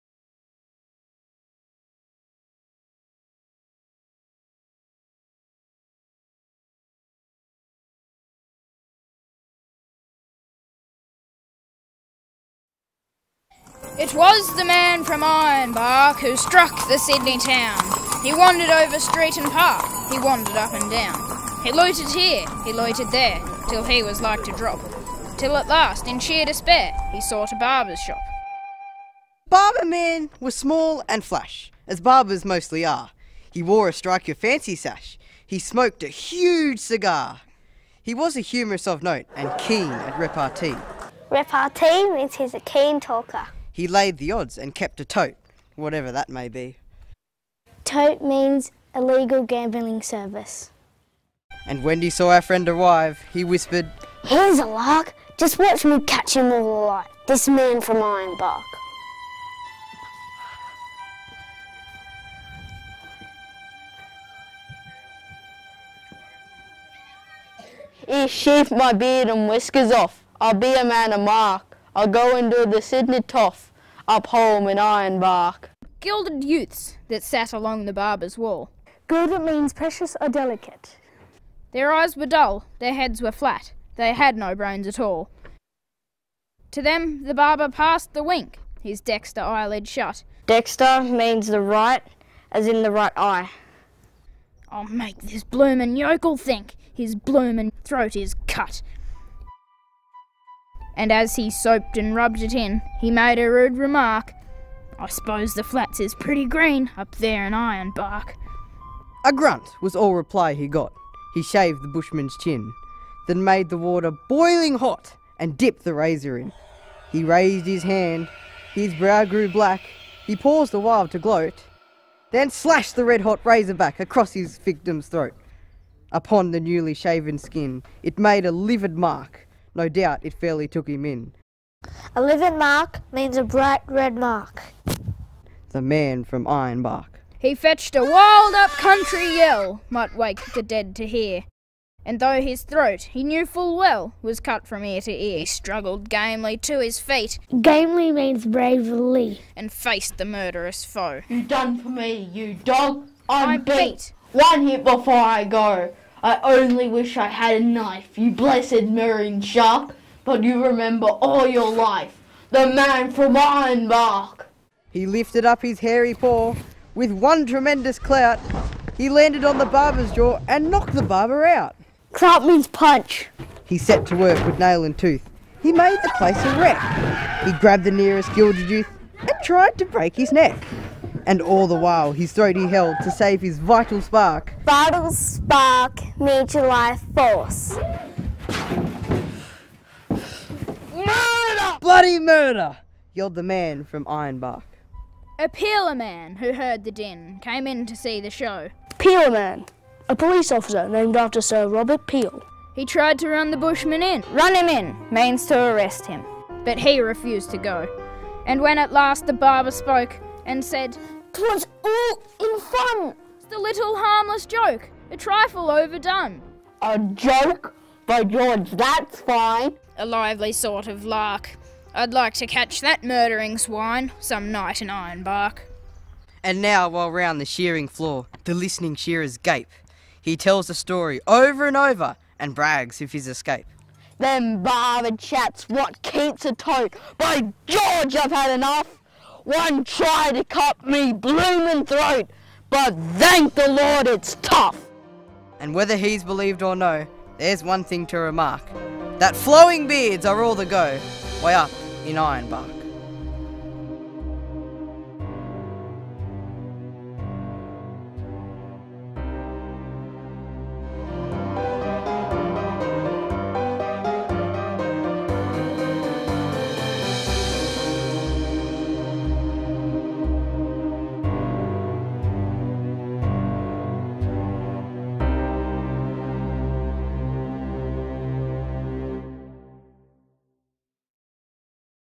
History Here: Man from Ironbark
Spring Hill Public School did an interpretation of the Man from Ironbark. “The link according to Principal McAnulty is that Banjo Paterson is buried down the road so they own him,” Paul Stafford said.
One of five short films about Orange region’s history, made by school students collaborating with local seniors, will screen at the Amusu Theatre in Manildra on Saturday 10 June, 2017.
History Here is an Arts OutWest project, devised and led by author and literacy consultant Paul Stafford. Additional editing by Vince Lovecchio.
Funded by NSW Office of Environment & Heritage’s Community Youth and Seniors Heritage Grants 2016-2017.